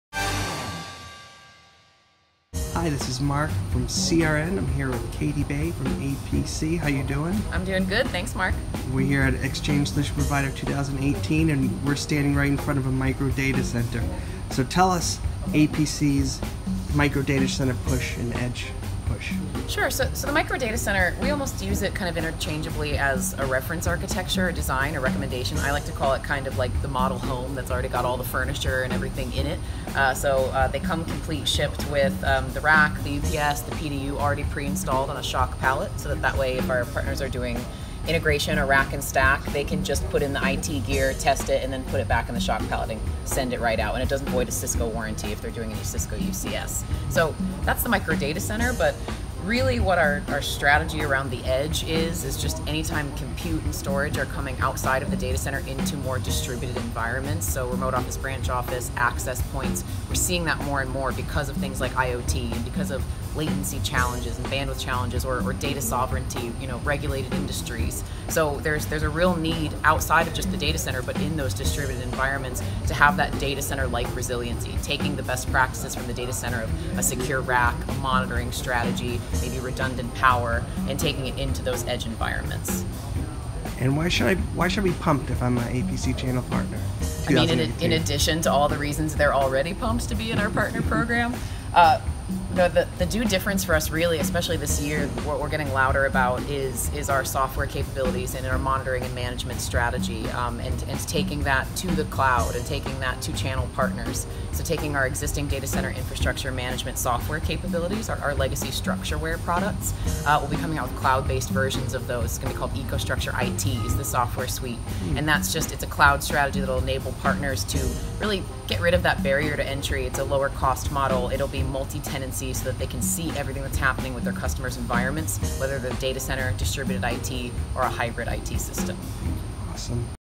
APC by Scheider Electric ‘Getting Louder’ About Software, New Cloud Solutions
There's a real need to have data-center like resiliency into edge environments, according to APC's Katie Boeh.